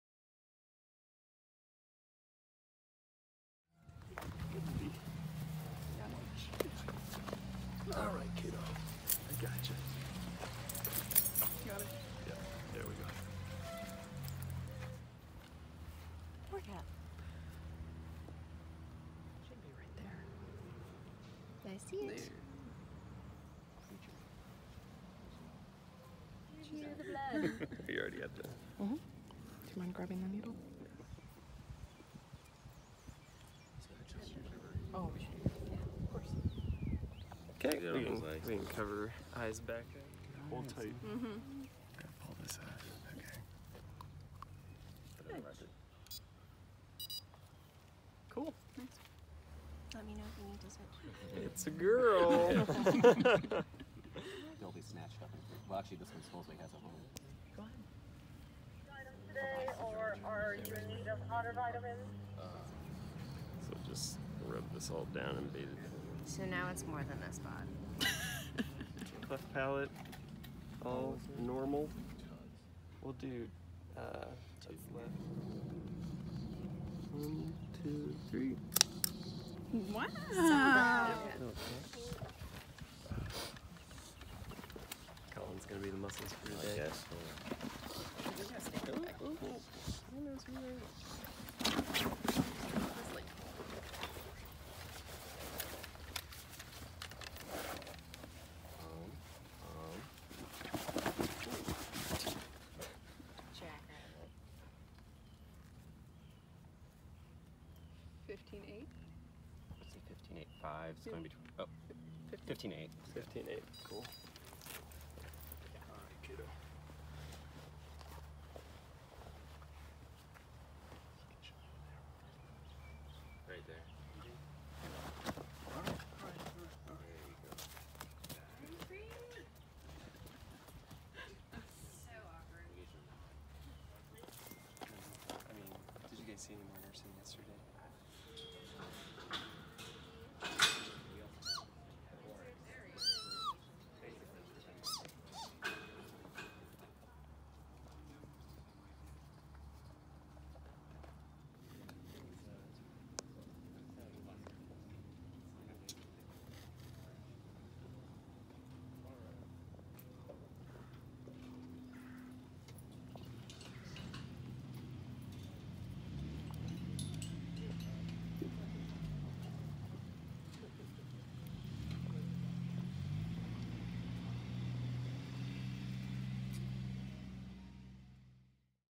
Sable Baby Neonatal Exam
On April 23rd, a female Sable antelope was born here at Zoo Miami, weighing 35lbs! She later had a neonatal exam, which we captured in this video that included an overall health check, as well as the collecting of blood, insertion of a microchip, notching of the ear, and weighing. Make sure to watch until the end when she's released back into the pen and then calls for her mother with the cutest little “squeaks” you'll ever hear from any antelope!